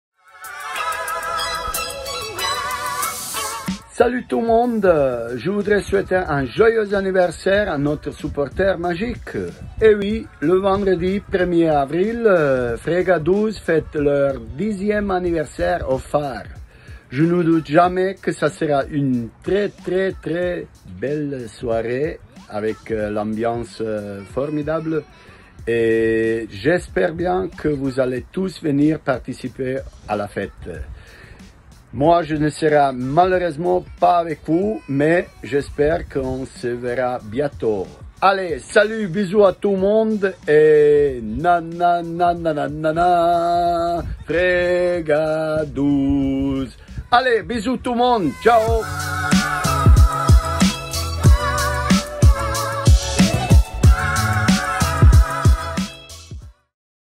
10 ans de la Frega 12 - Un message d'Edin Basic
À l'occasion de l'anniversaire de la Frega12, les joueurs Chambériens ont un message pour le public 'Jaune et Noir' !